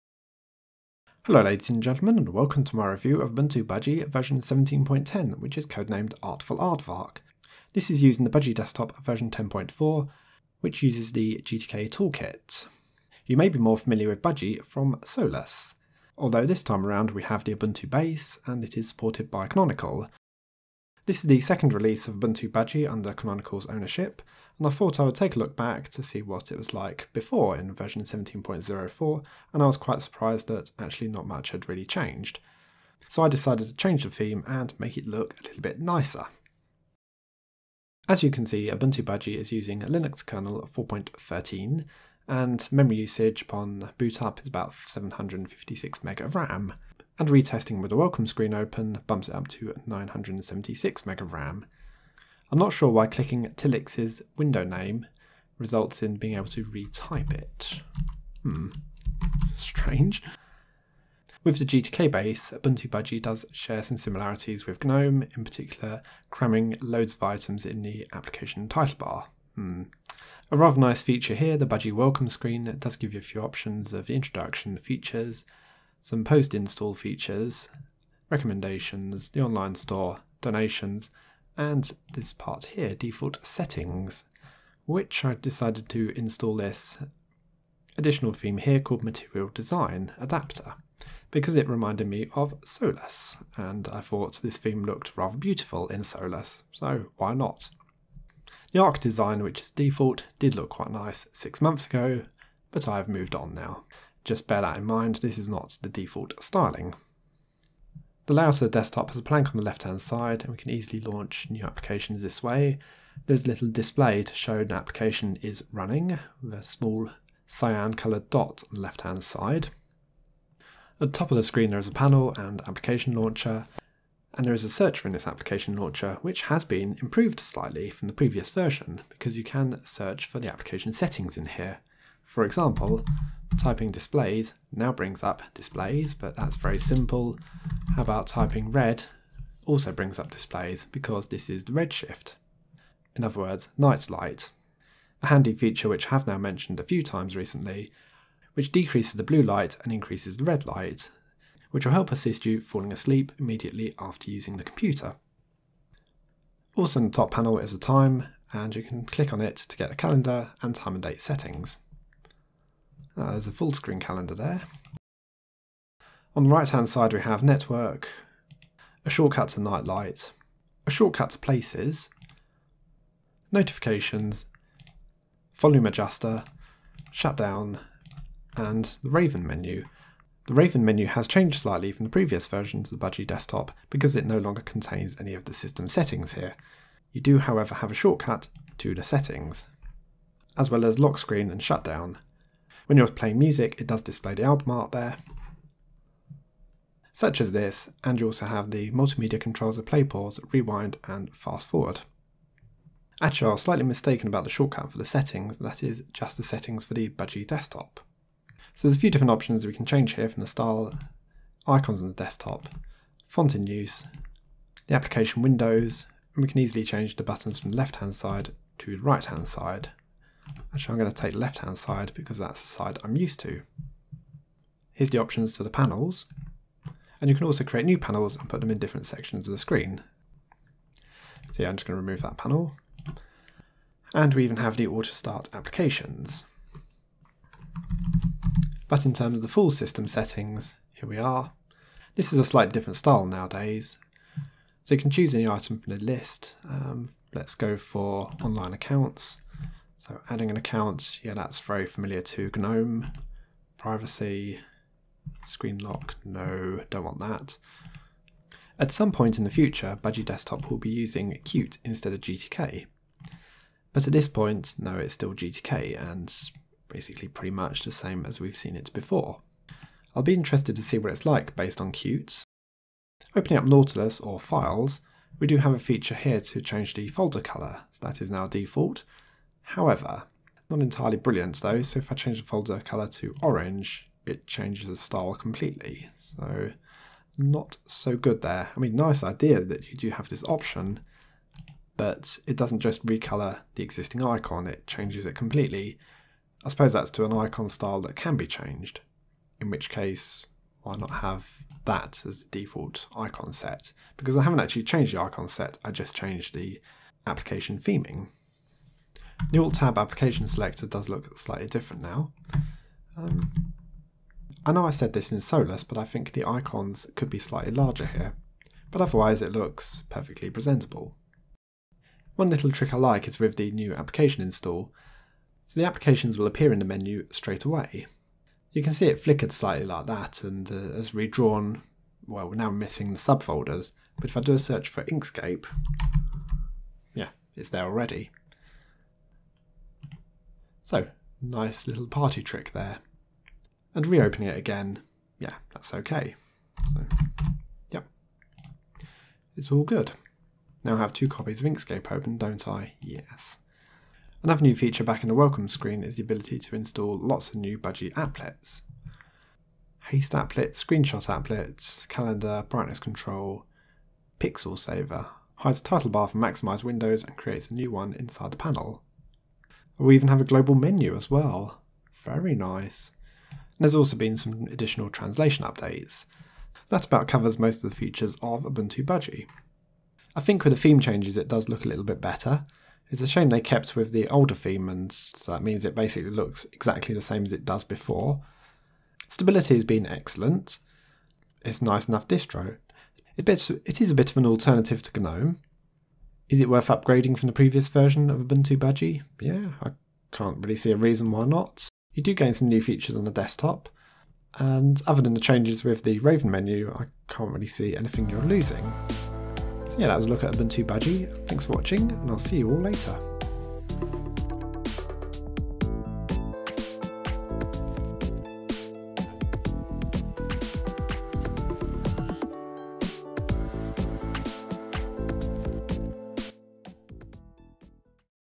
Ubuntu Budgie 17.10 Review
Budgie Desktop started out life in Solus, but is now available as an official derivative in Ubuntu Budgie.
Ubuntu Budgie comes with the Budgie desktop v10.4 which is still based on GTK.
Out-of-the-box it doesn’t look as pretty as Solus, although that can be rectified by changing theme. Stability has improved slightly from previous release